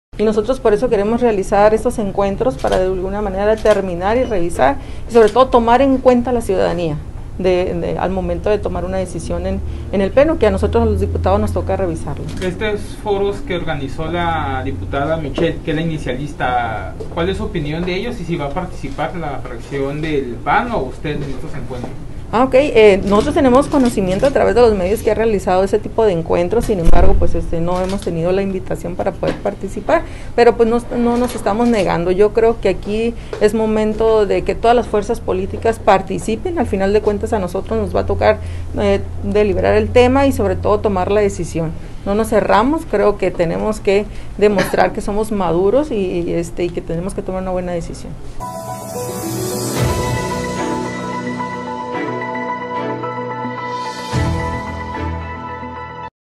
CNR Mexicali diputados del pan no han sido requeridos en foros sobre el aborto realizados por morena
Participarán diputados del PAN en encuentros sobre la despenalización del aborto independientes a los organizados por la diputada morenista, Michelle Sánchez, ya que no fueron invitados a estos. Amintha Briceño, legisladora blanquiazul, lo informó…